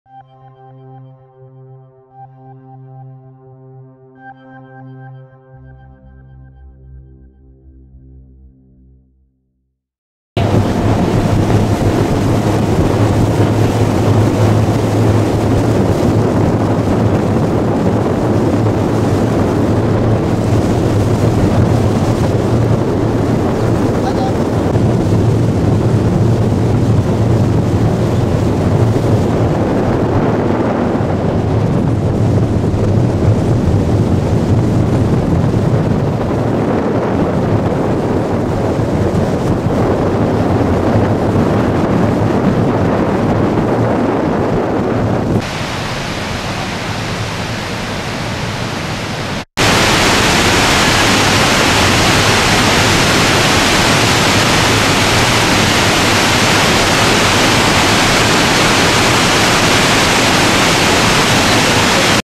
On Top of Victoria Falls Zambia
We've all seen the cartoons in which, much to his dismay, the protagonist in a canoe approaches an  unexpected  waterfall.  This is what it like to approach Victoria Falls, the largest waterfall in the world, from behind on the thin sheets of water in the Zambezi River on the top of the flat basalt ledge next to Livingstone Island in Zambia, just before the breathtaking 108 meter (354 ft) fall from the top of the cliff.